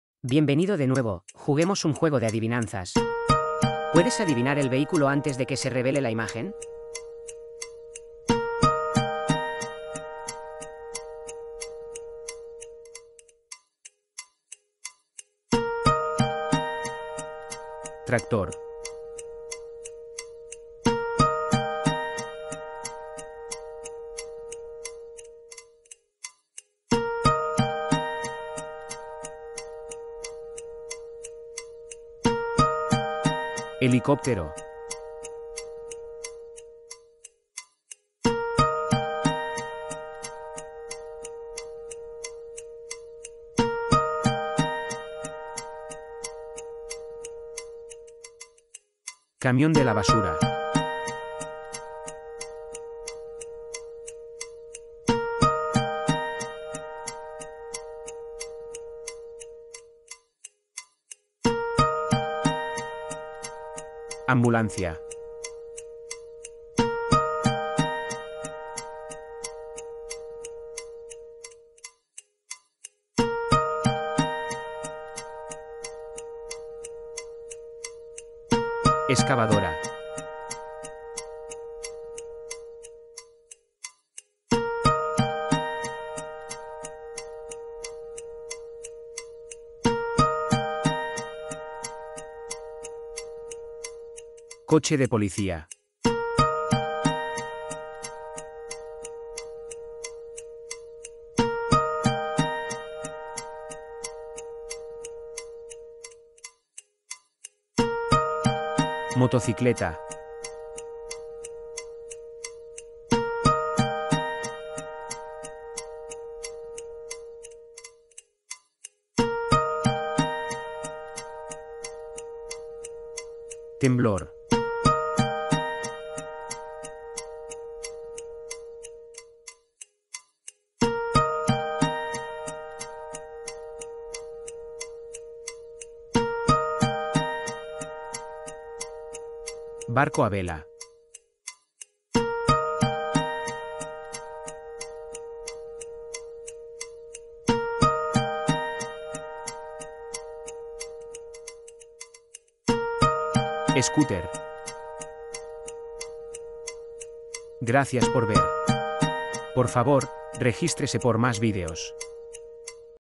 Adivina el vehículo | juego de preguntas
bienvenido de nuevo! Juguemos un juego de adivinanzas :) ¿Puedes adivinar el vehículo antes de que se revele la imagen?